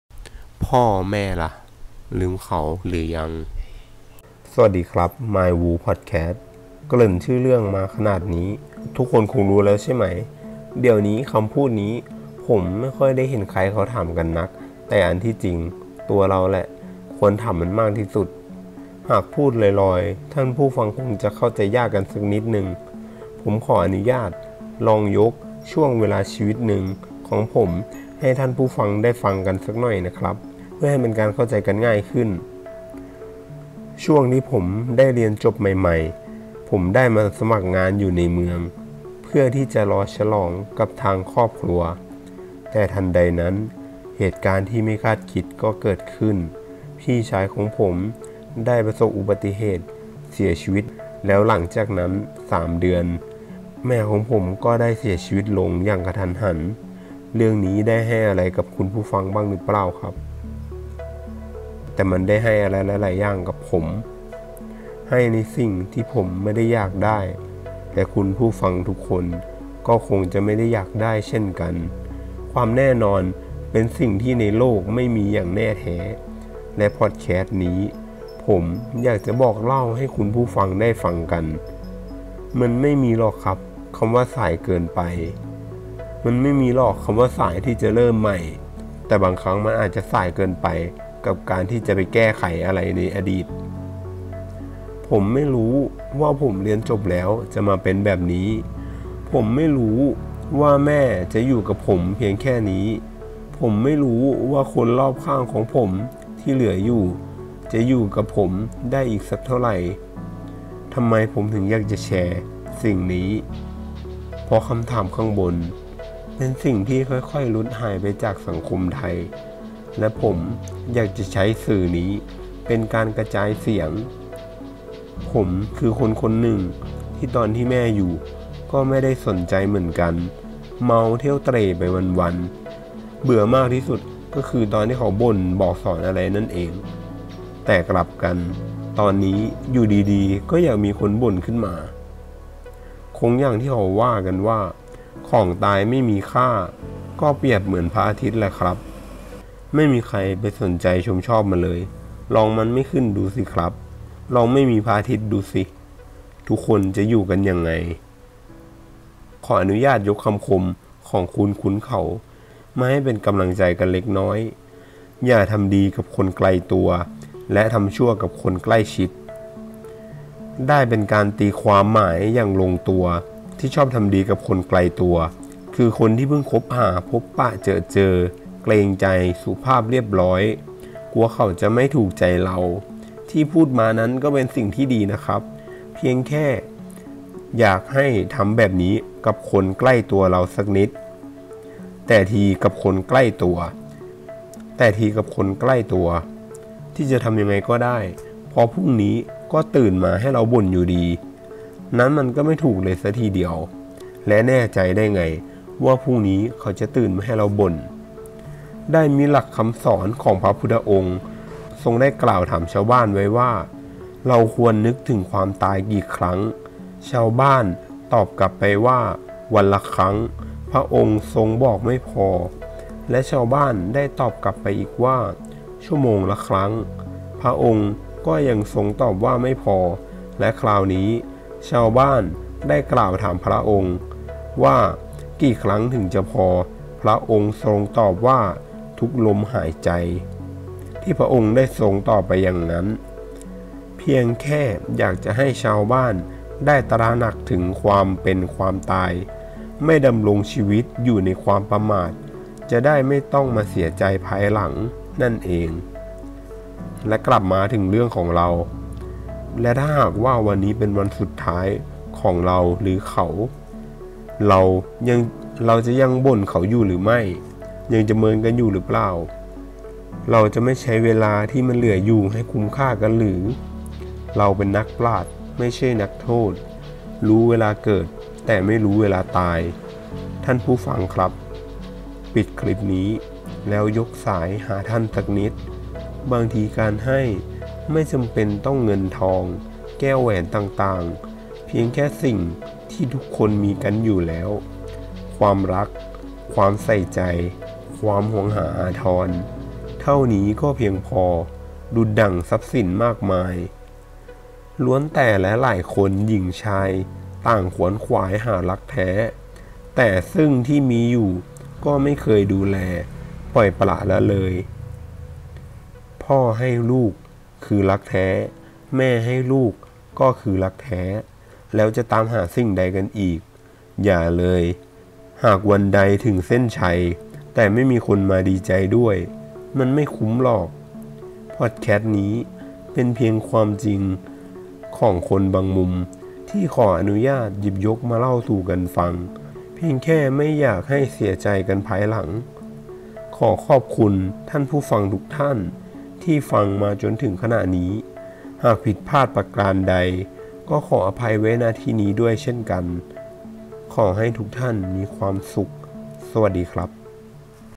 "พ่อแม่" ลืมเขาหรือยัง?
คำถามบางคำถาม เป็นของคนอื่น จะมีสักกี่คำถาม ที่เราควรตั้งให้ ตัวเองตอบ จะเป็นคำถามใดนั้นที่เราควรตอบ เรามาฟังไปพร้อมๆกันเลยครับ

- ขอให้ทุกท่านมีความสุข แด่ความฝันอันสวยงาม.

I don’t want a perfect life; I want a happy life